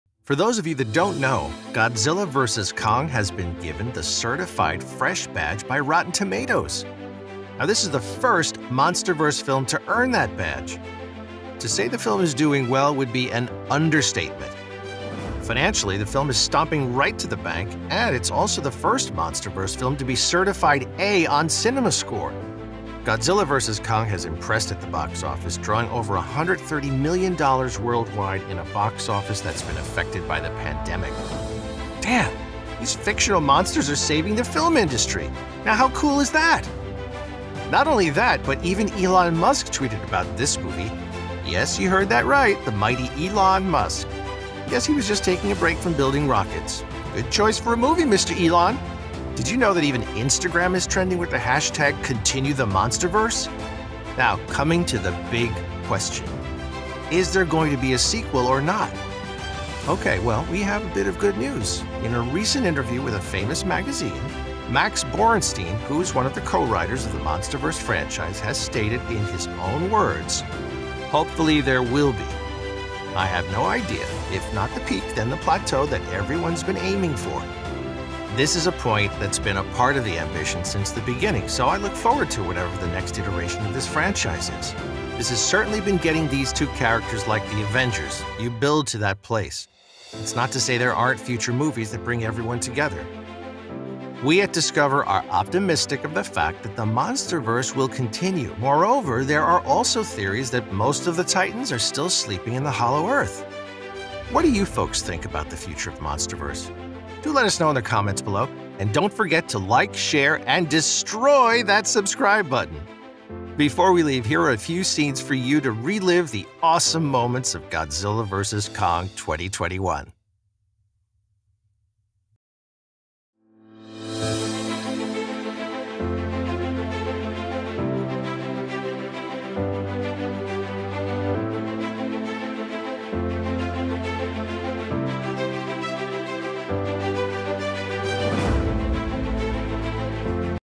Elon Musk/ New Godzilla vs Kong 2021 sequel??
Watch this video to find out if we are in for another sequel or if the monsterverse will continue?